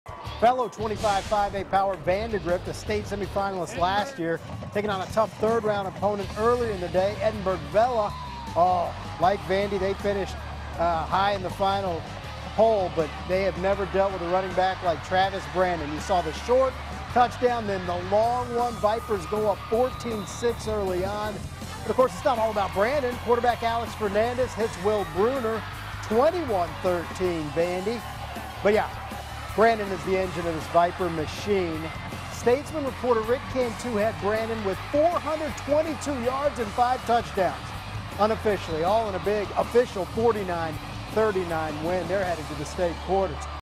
High School Football Playoffs Week 3 - Vandegrift vs Edinburg Vela
Vandegrift takes on Edinburg Vela.